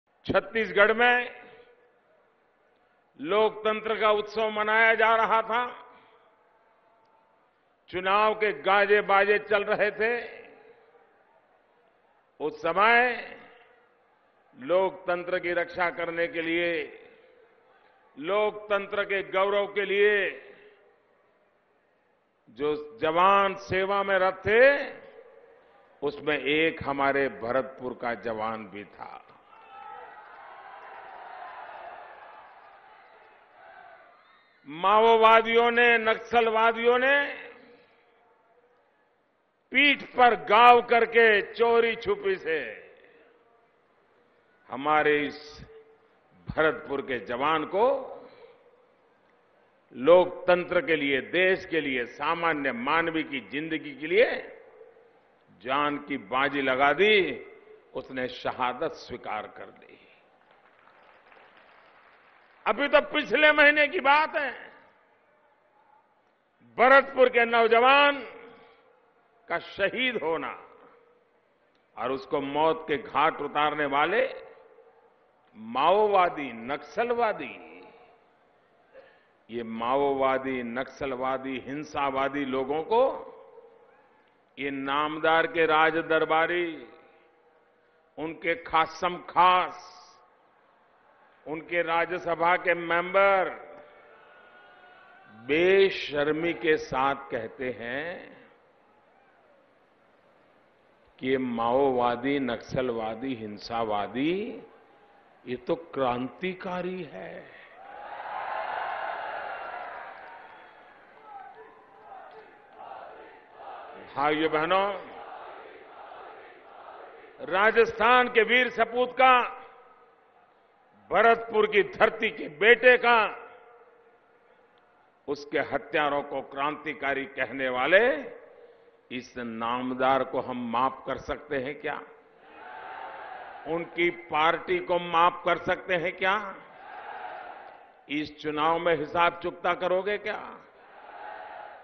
PM Modi addresses Public Meeting at Bharatpur, Rajasthan | शहादत का अपमान कर रहे हैं
PM Modi addresses Public Meeting at Bharatpur, Rajasthan | भरतपुर के लाल की शहादत का अपमान कर रहे हैं